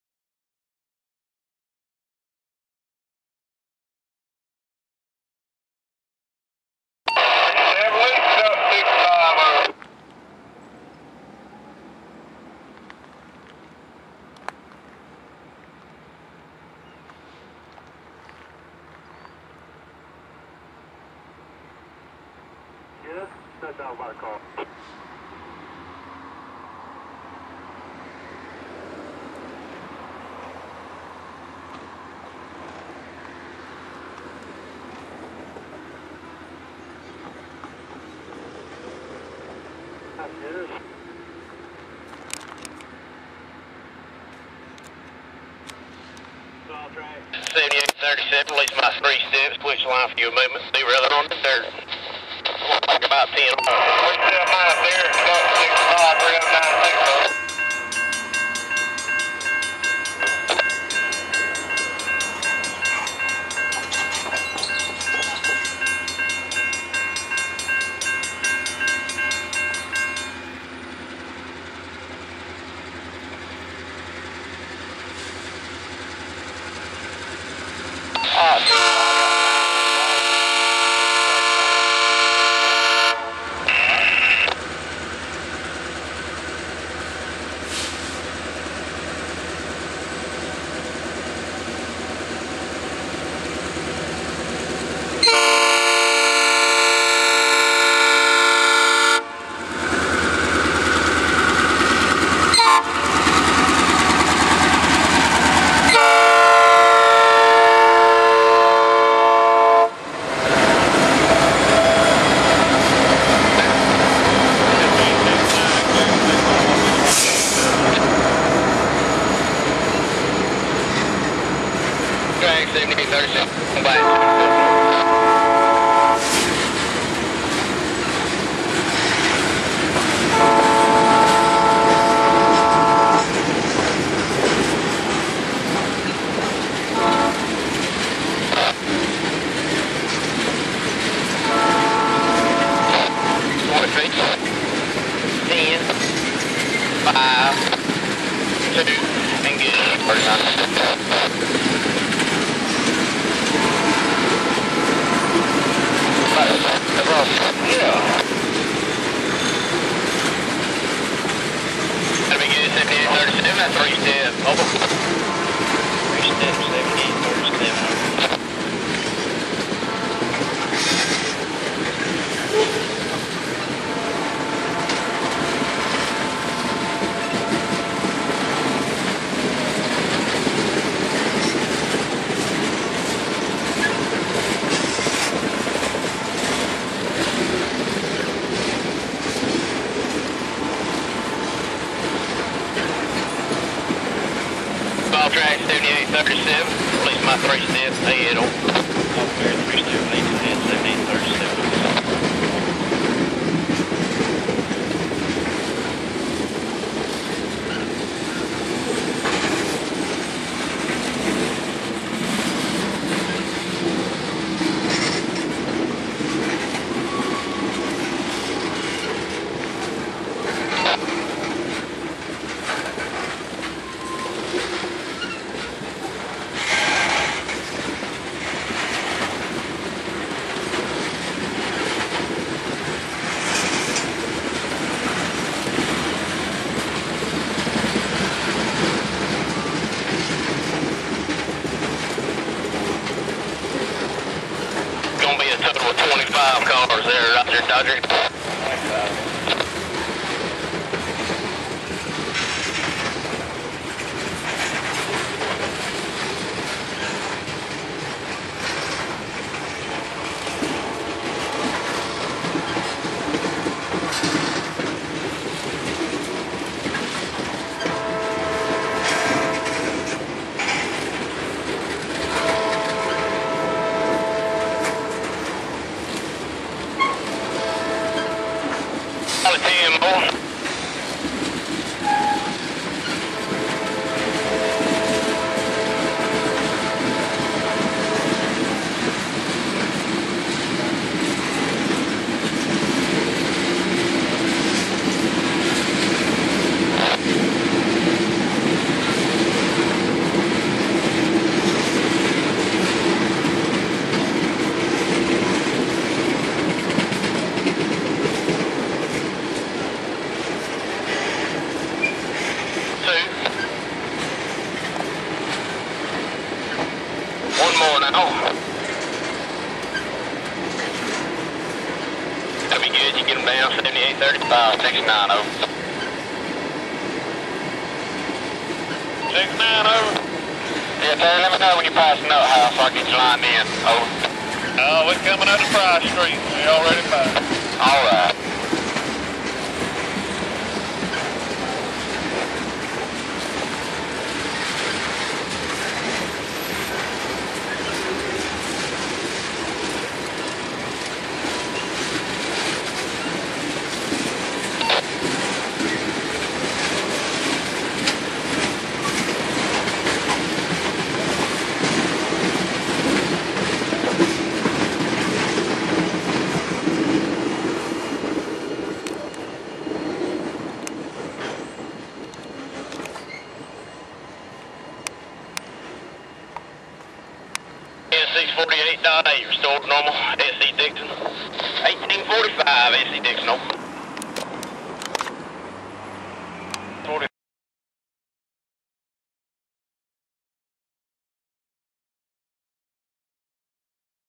Norfolk Southern G-69
Mp 151.4G...Oak St...Valdosta,GA
September 29, 2011...6:42 pm
AAR Mark: NS G-69
Origination: Norfolk Southern Langdale Yard...Valdosta, GA
Destination: PotashCorp Inc. White Springs, FL (turn around)
Molten Sulfur, Phosphate, Phosphoric Acid
Frequency: Daily

Norfolk Southern 5170...GP38-2...Ex-Southern 5170
Norfolk Southern 7054...GP50...Ex-Southern 7054